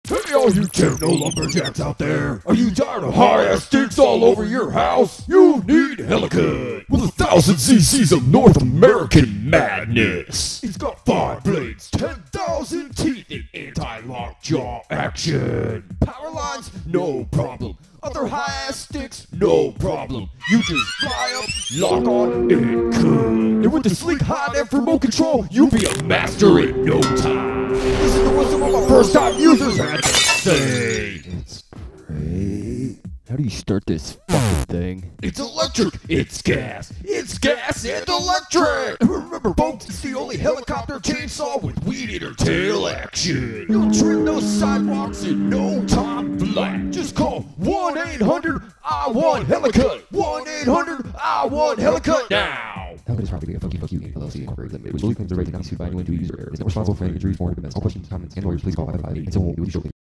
HELI CUT
i told sum idiot to hire a pro to cut a tree down.
made video.
he hired a junkie.
tree fell on house .
almost killed his wife.
moron's!